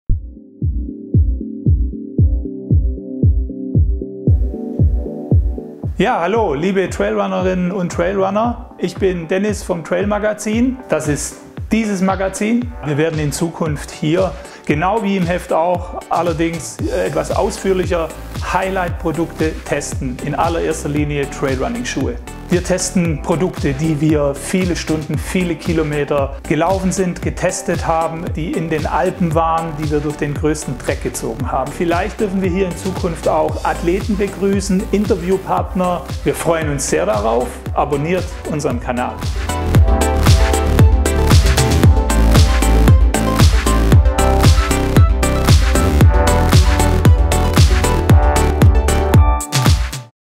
Intro. Der neue TRAIL Magazin Channel
Das TRAIL Magazin erweitert sich um eine Plattform und wir künftig hier Highlightprodukte vorstellen und testen, mit Interviewpartnern sprechen und einiges mehr rund um Trailrunning in bewegte Bilder bringen.